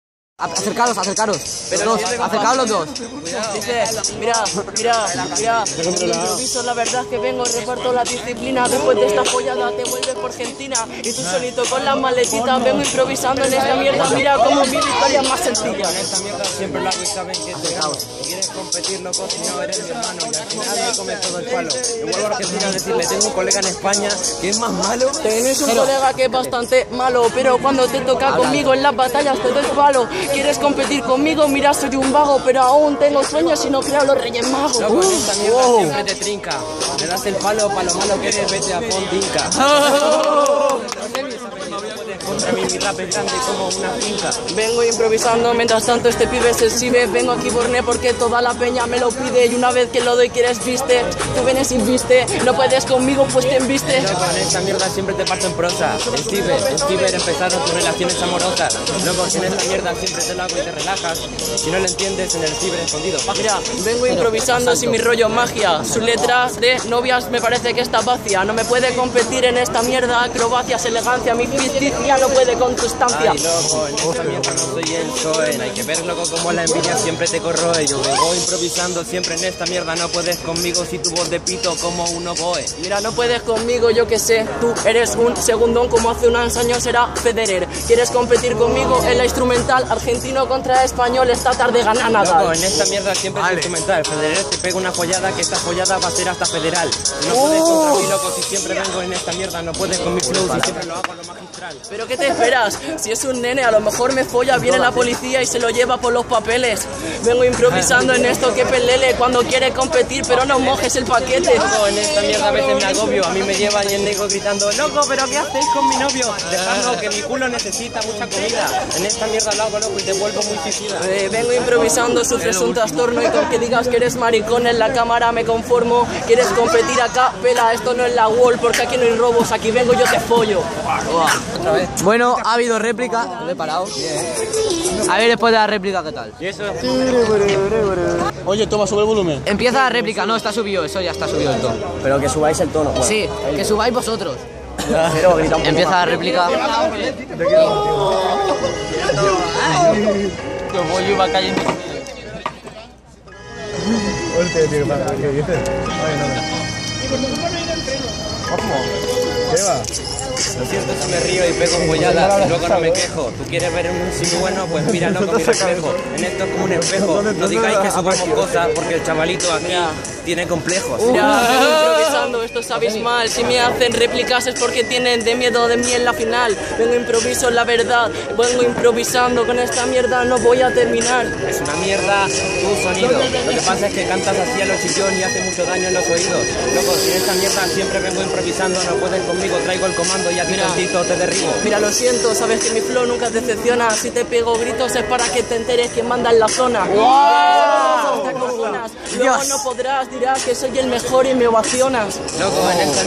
JERO VS NEYKO BATALLAS BORNE
Hay dos replicas, acaba ganando Jero